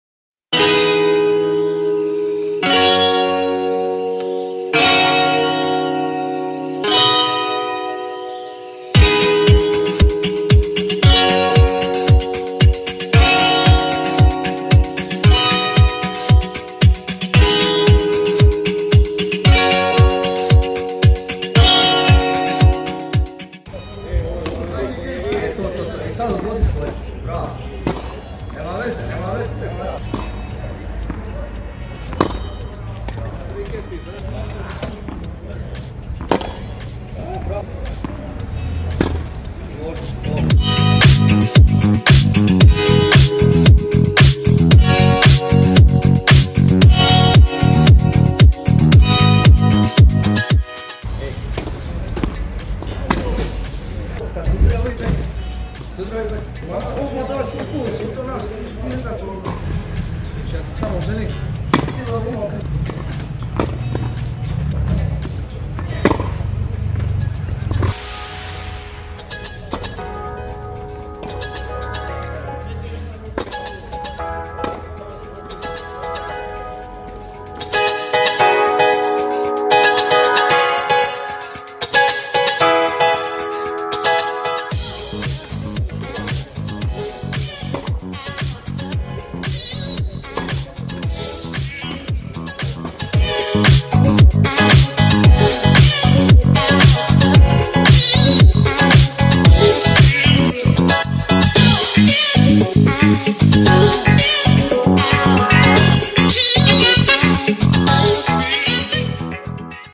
TKOI SPLIT 2020 - Transfering Wheelchair Tennis Knowledge To New Player Ante
Teaching fresh player some movement and techinque tips.